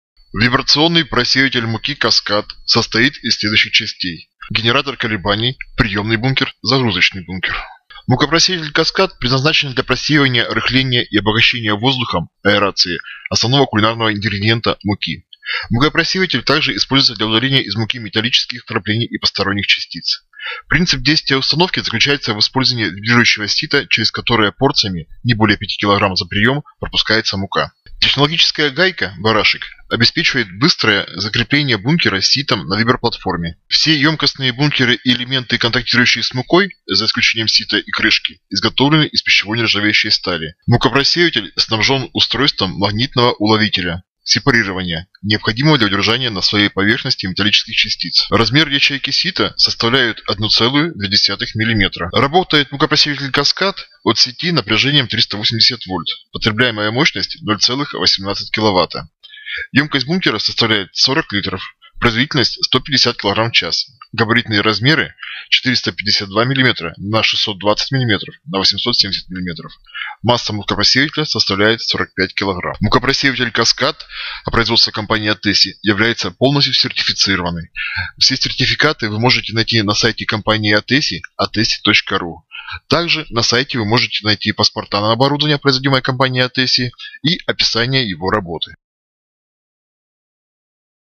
мукопросеиватель Каскад от ПРАЙД ГРУПП
Компания ПРАЙД ГРУПП производит кухонне оборудование. На всех этапах производства "ПРАЙД" осуществляет контроль качества. Каждая партия проходит экспертизу качества.